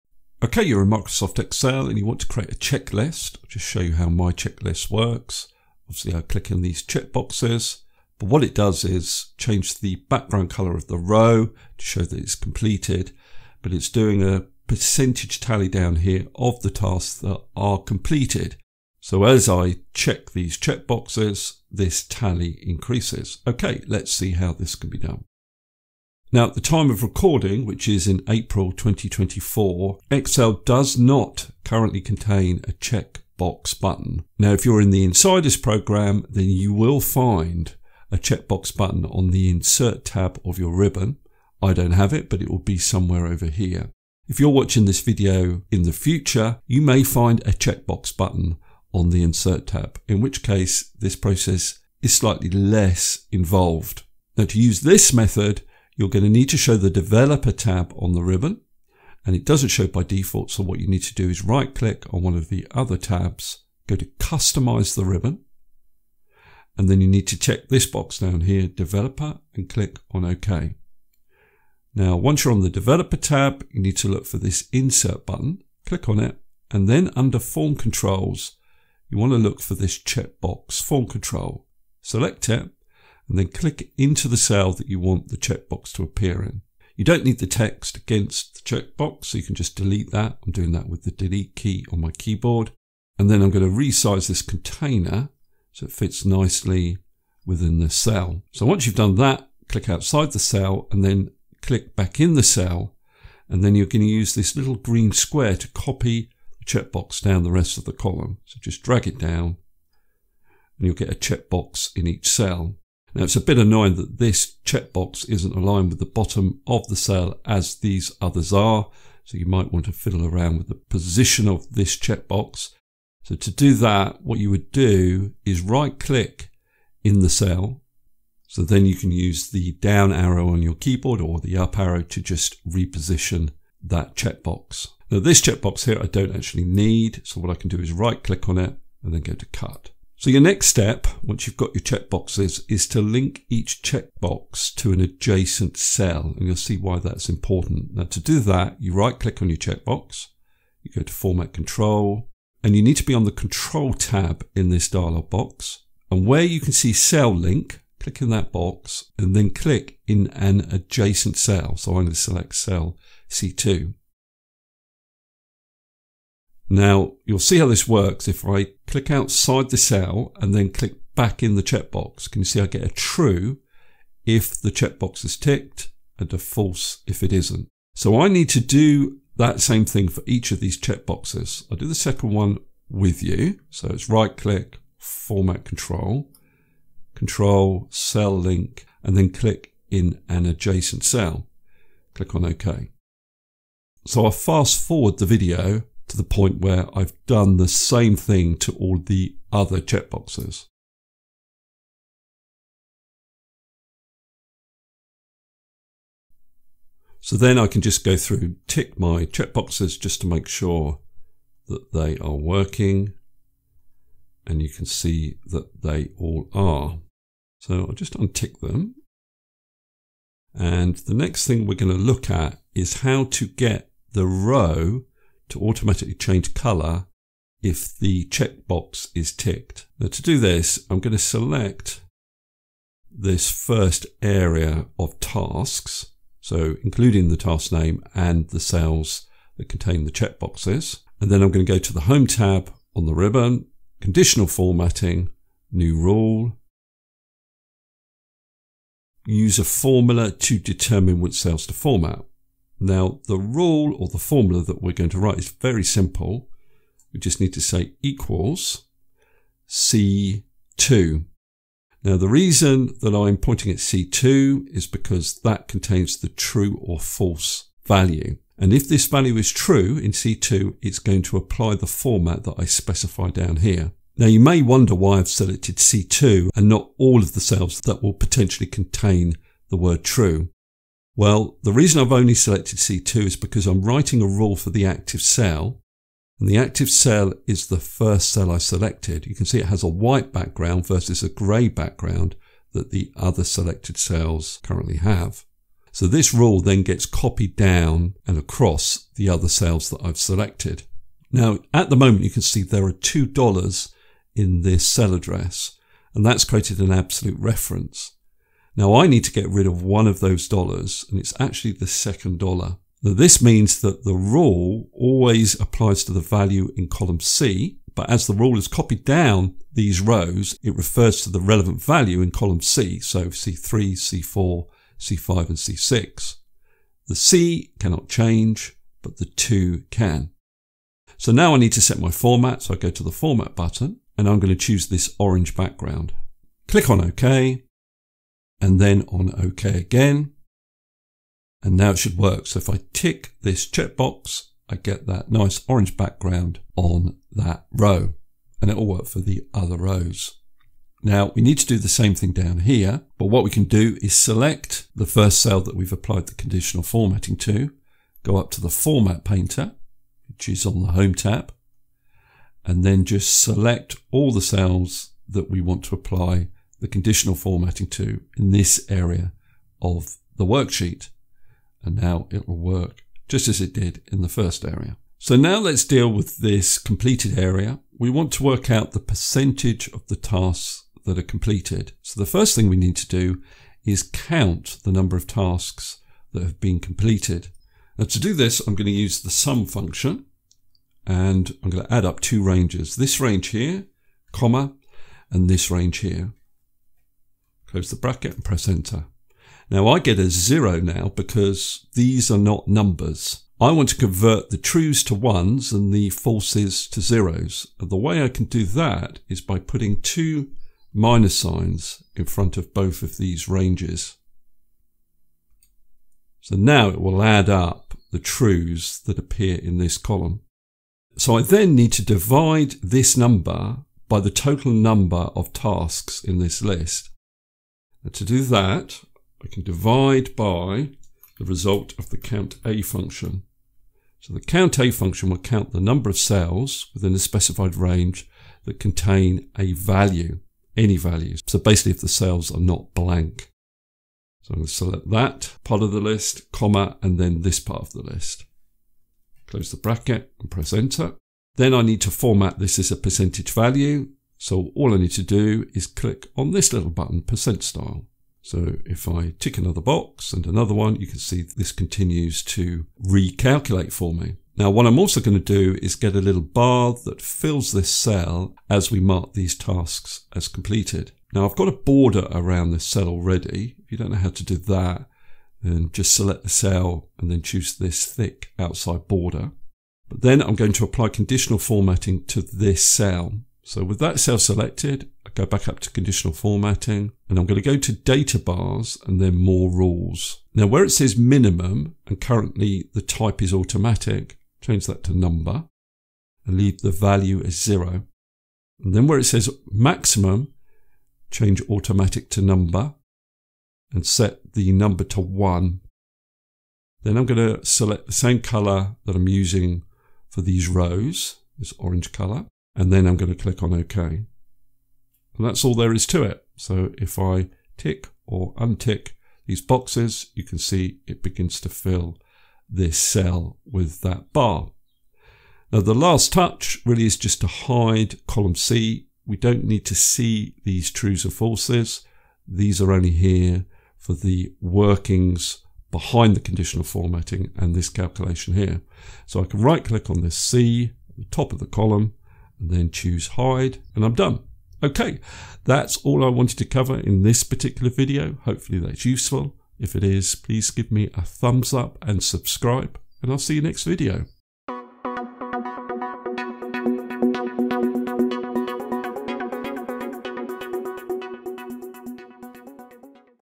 How to Create a Checklist in Excel | Interactive Checklist Template with Checkboxes
In this Microsoft Excel video tutorial I explain how to create a checklist in Excel.

🕰️ Timestamps
00:00 Introduction
00:26 Insert checkboxes
03:39 Conditionally format rows where task is complete
06:14 Perform a % complete calculation